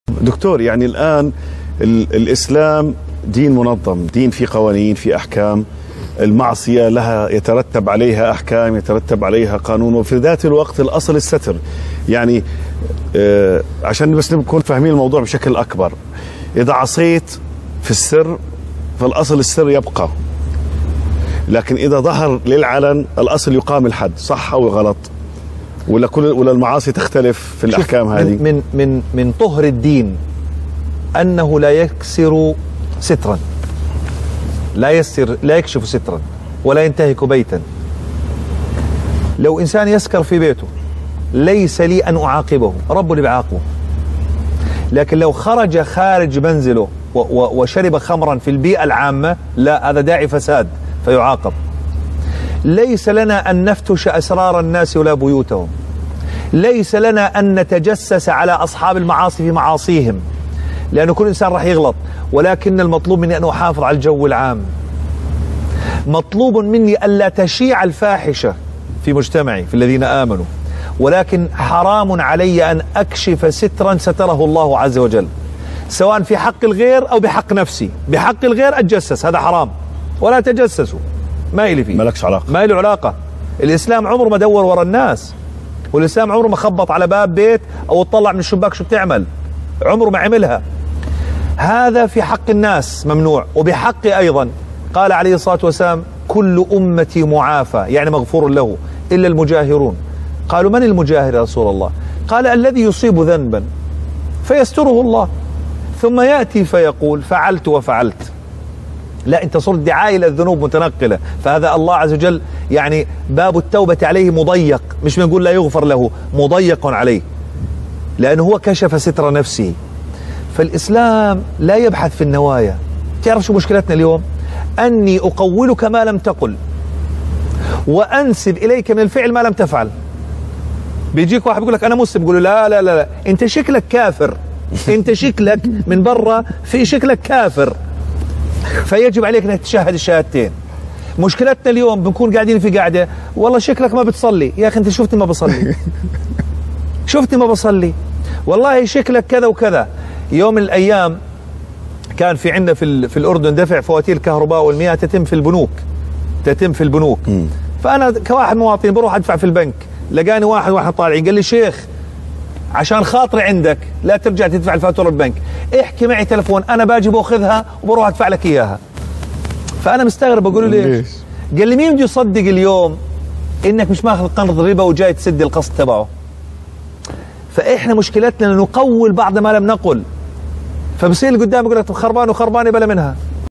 لا تتجسس على صاحب المعصية
مع د. محمد نوح القضاة في المرآة
تابعوا قناة إقرأ على الفيسبوك باللغات التالية: